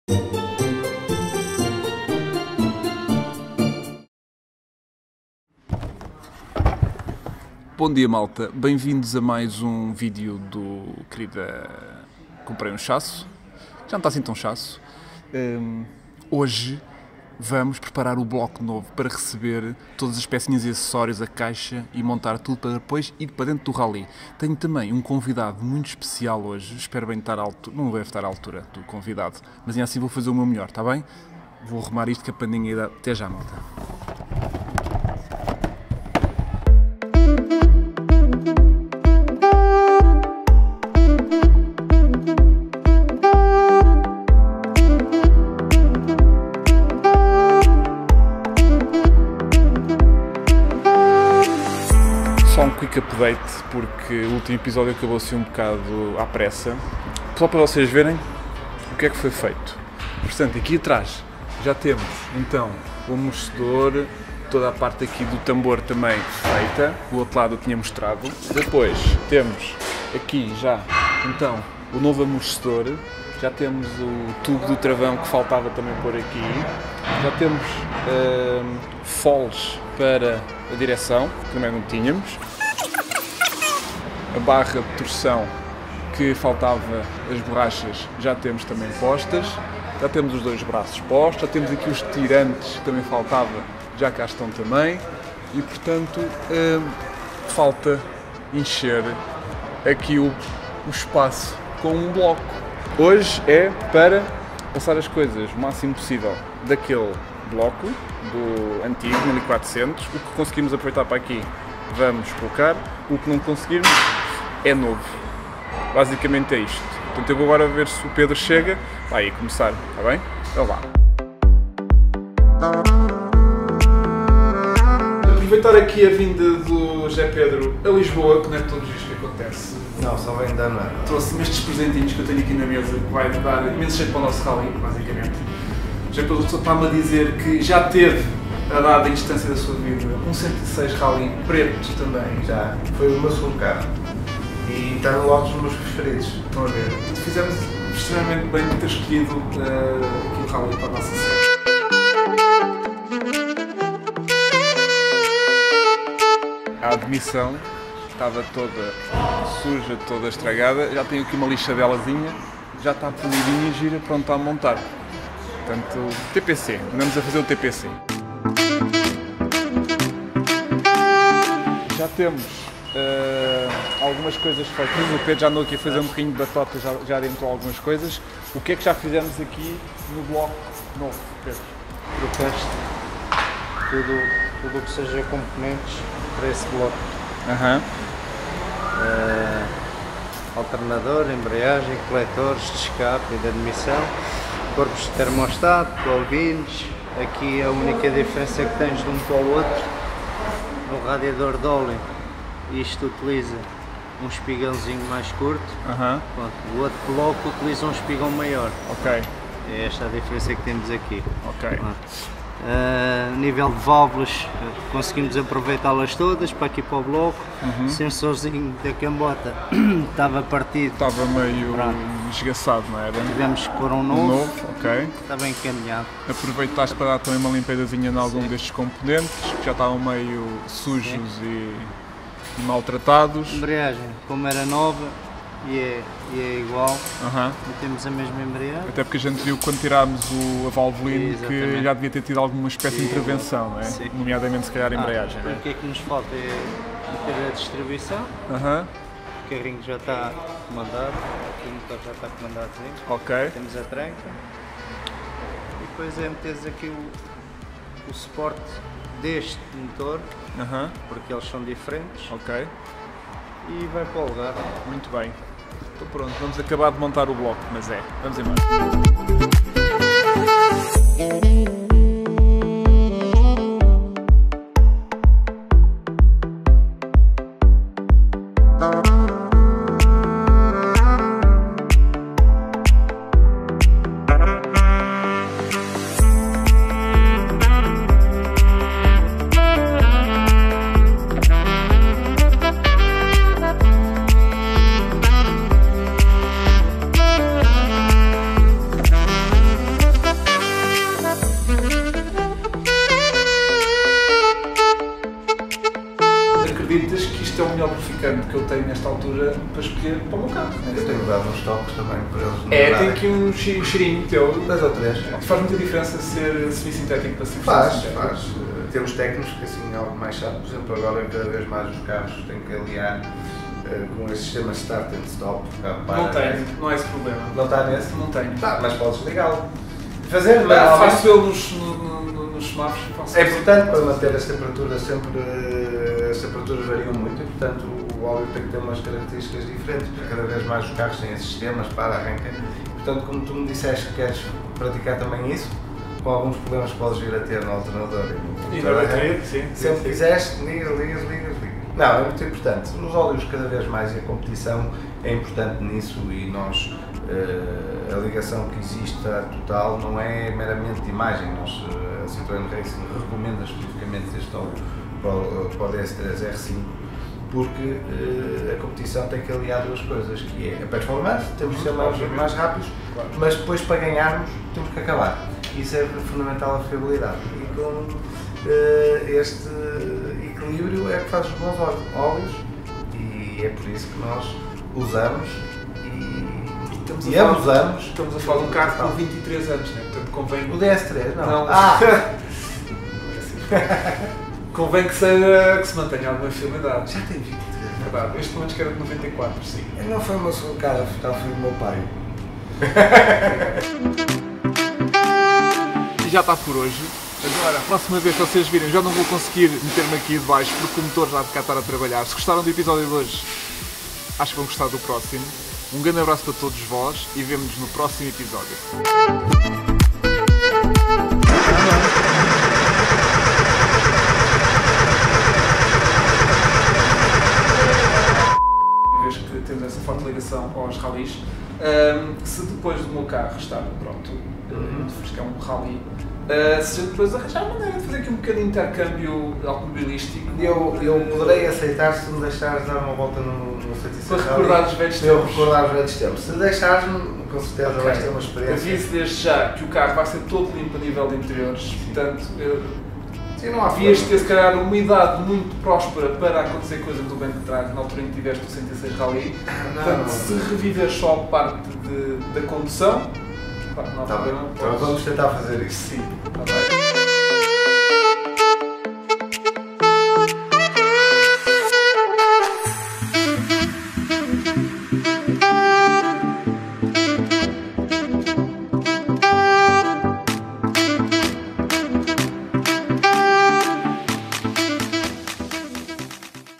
FINALMENTE MOTOR 1.3 do 106 RALLYE PRONTO | Querida, Comprei um Chaço #6
Querida, Comprei um Chaço | Ep. 6 - MOTOR PRONTO!

Finalmente o motor está pronto para ser montado no 106 Rallye!